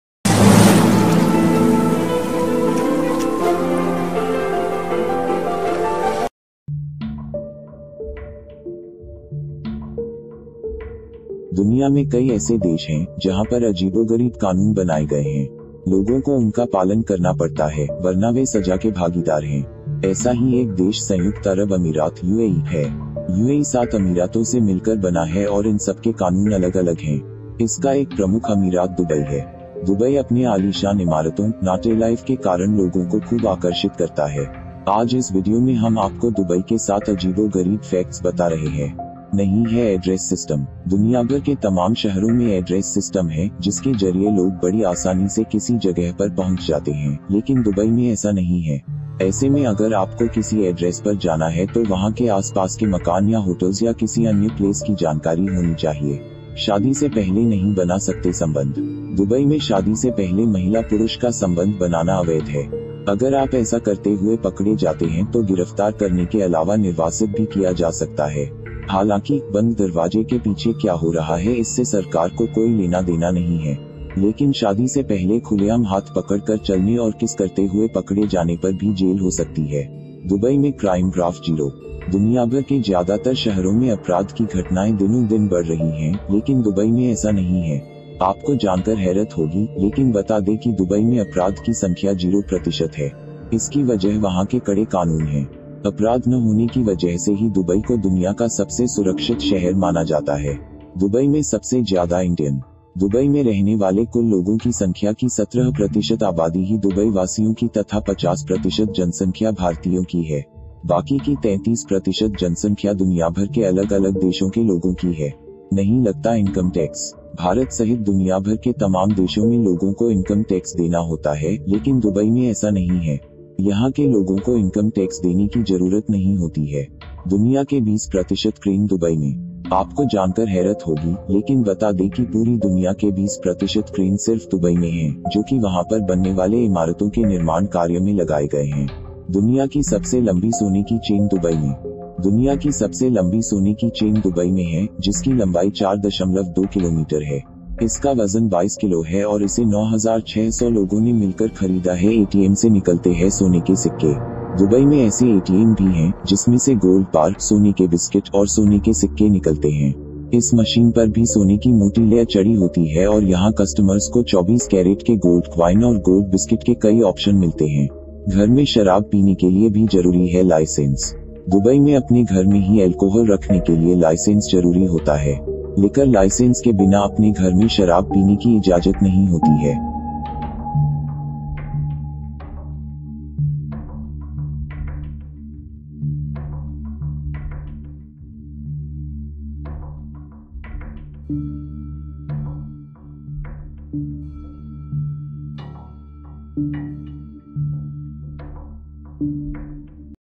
दुबई में रहना है तो इन कानूनों को ज़रूर जान ले...
दुनिया में कई ऐसे देश हैं, जहां पर अजीबोगरीब कानून बनाए गए हैं। लोगों को उनका पालन करना पड़ता है, वरना वे सजा के भागीदार हैं। ऐसा ही एक देश संयुक्त अरब अमीरात (UAE) है। यूएई सात अमीरातों से मिलकर बना है और इन सबके कानून अलग-अलग हैं। इसका एक प्रमुख अमीरात दुबई है। दुबई अपने आलीशान इमारतों, नाटइलाइफ के कारण लोगों को खूब आकर्षित करता है। आज इस लेख में हम आपको दुबई के 7 अजीबो-गरीब फैक्ट्स बता रहे है।